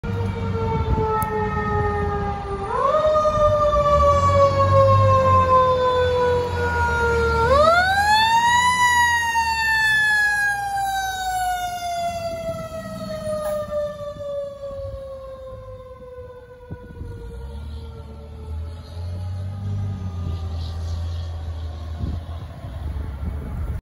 Wauwatosa WI Fire Dept. Engine 51 Responding
Tosa Engine 51 responding to a medical emergency.